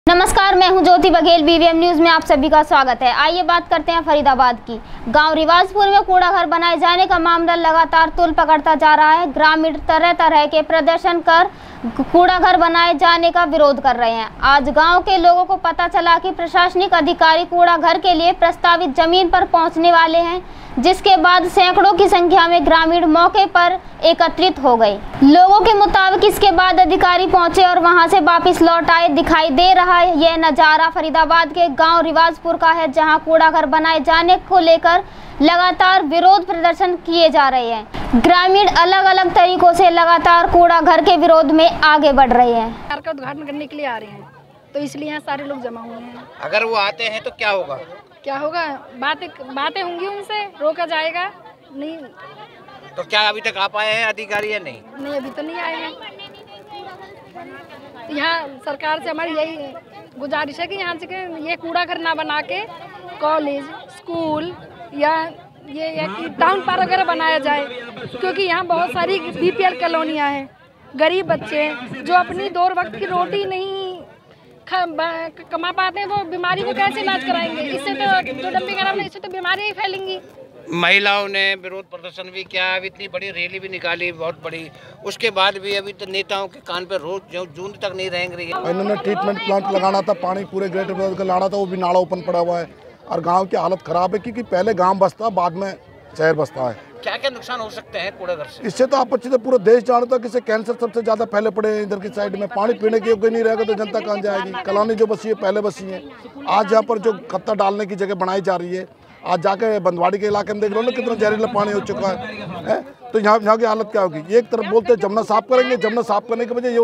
गांव में कूड़ा घर मामला लगातार तूल पकड़ता जा रहा है
bvmnews haryananews topnews bvmnewslive latestnews ║गांव में कूड़ा घर मामला लगातार तूल पकड़ता जा रहा है